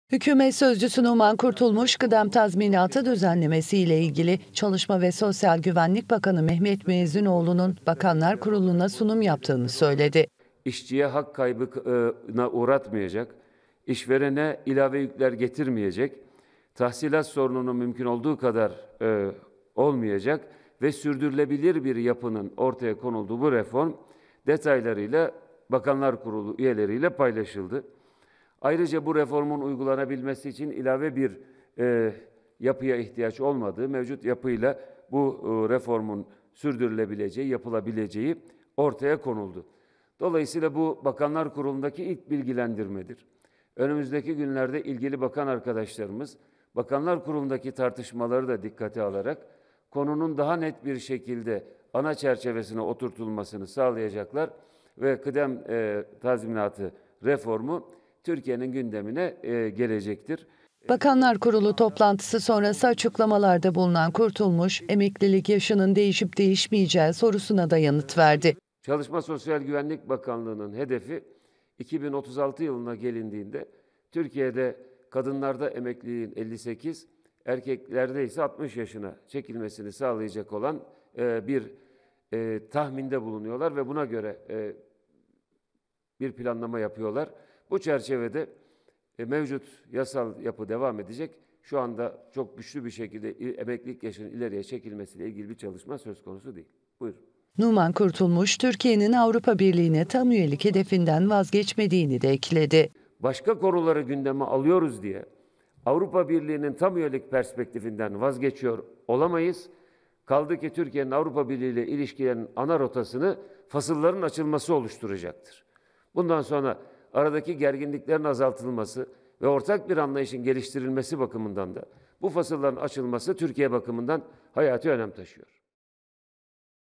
Hükümetten ’kıdem tazminatı ve emeklilik yaşı’ açıklaması
Başbakan Yardımcısı Numan Kurtulmuş, emeklilik yaşının yukarıya çekilmesine ilişkin bir çalışmanın gündemde olmadığına işaret ederek, "Mevcut yasal yapı devam edecek. Emeklilik yaşının ileriye çekilmesi ile ilgili çalışma söz konusu değil" dedi.